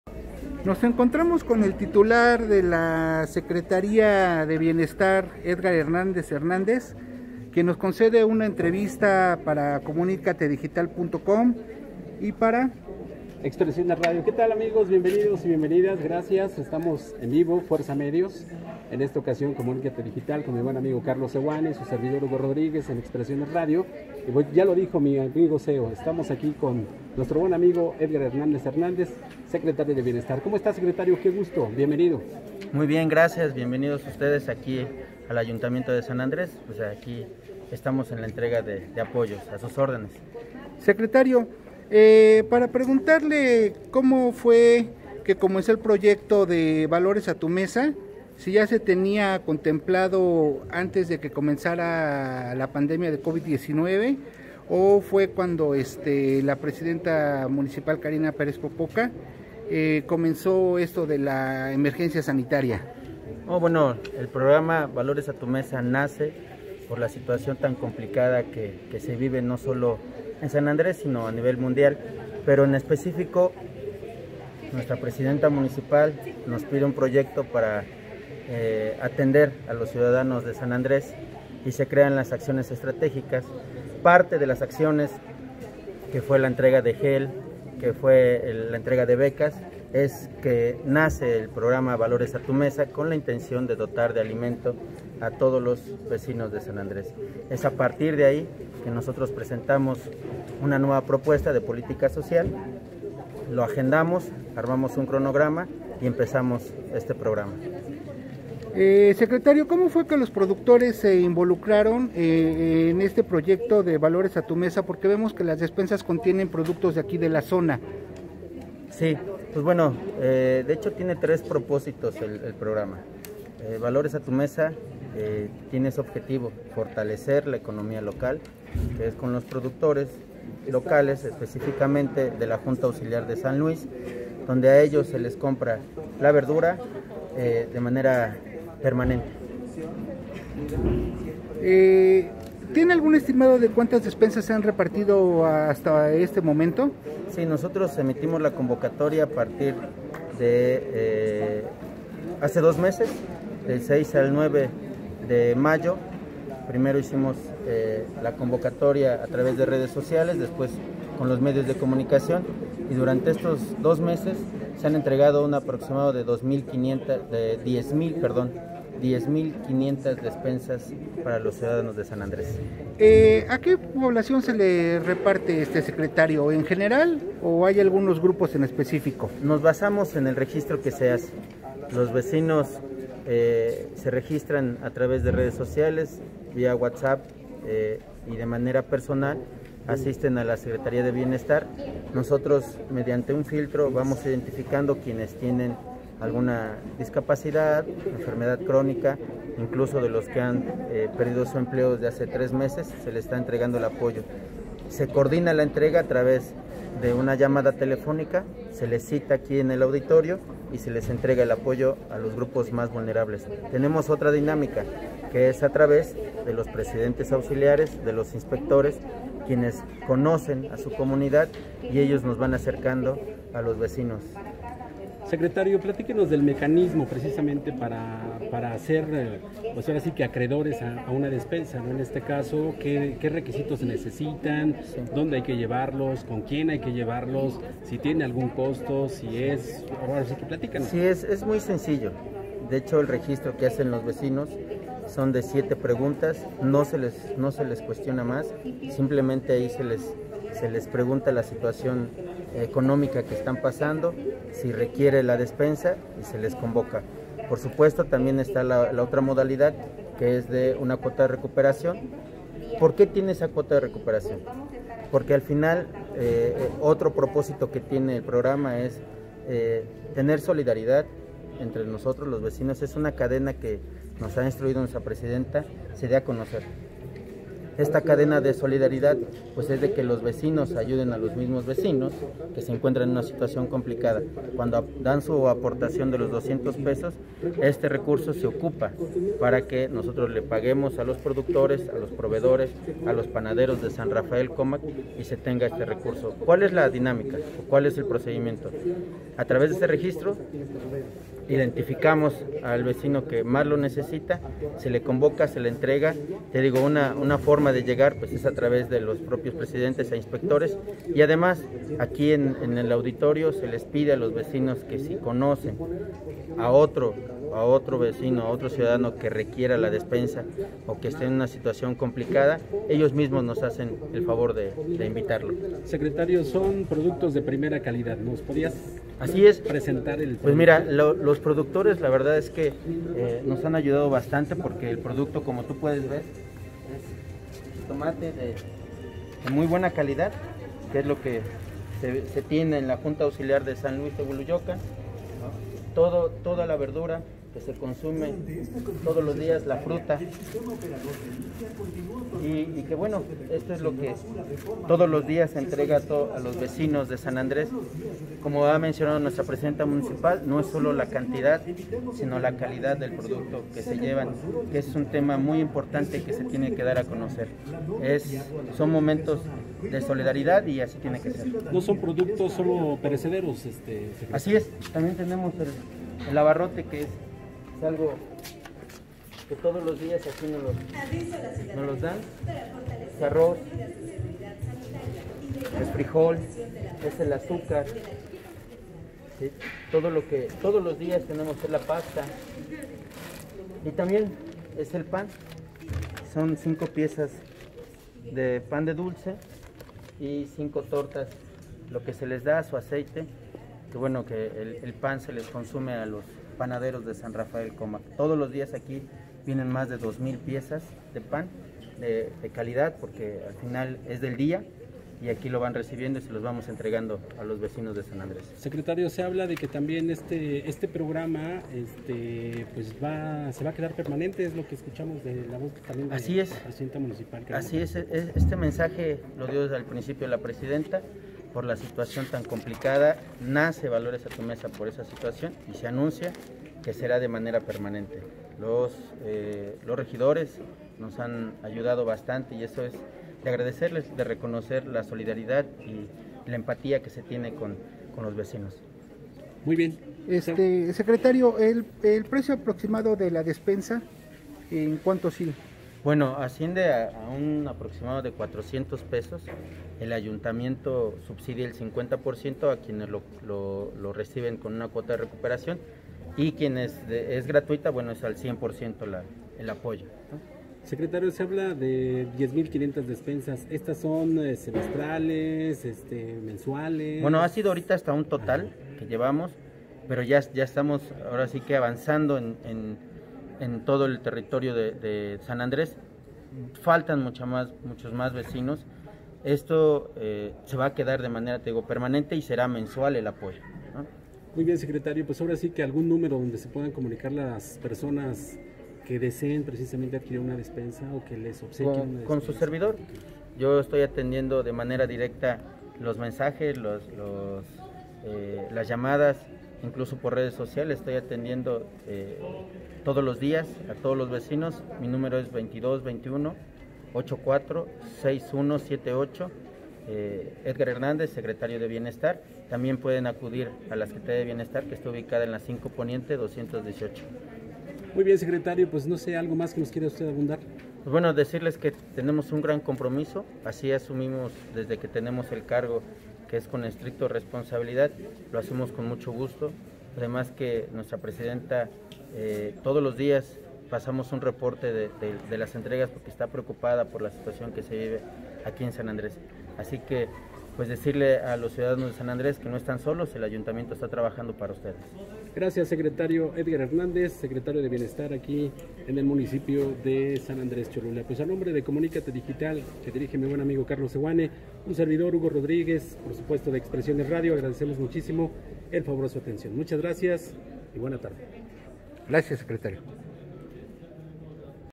Más de 10 mil 500 familias sanandreseñas beneficiadas con “Valores a tu Mesa”: Edgar Hernández
Edgar Hernández Hernández, titular de la Secretaría de Bienestar del municipio de San Andrés Cholula informó que más de 10 mil 500 familias han sido beneficiados con el programa “Valores a tu Mesa”, con la dotación del mismo número de despensas.

“Valores a tu Mesa”, es un programa de la administración de Karina Pérez Popoca que busca ayudar con alimentos a grupos vulnerables o desempleados por la pandemia de Coronavirus, además de fortalecer la economía local, debido a que las verduras se adquieren con productores de la región, informó en entrevista con Comunicatedigital.com.